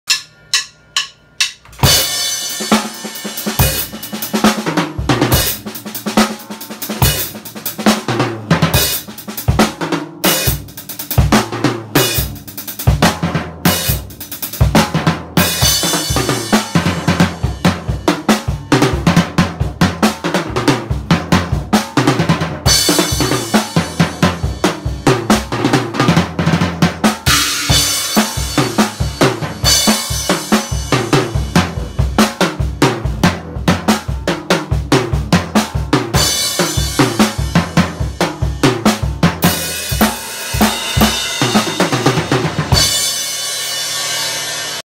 sweetbeatchallenge | KJDRUMS
Hey guys. Sorry I didn’t post yesterday. So here’s me doing @COOP3RDRUMM3R’s sweetbeatchallenge. I hope you guys enjoy.